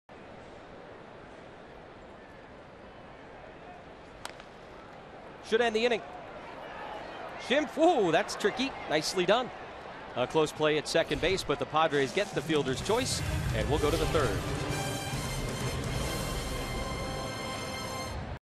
SD@WSH: Schimpf makes a nice toss with his glove
Ryan Schimpf fields a grounder from Danny Espinosa and tosses the ball to second with his glove for the forceout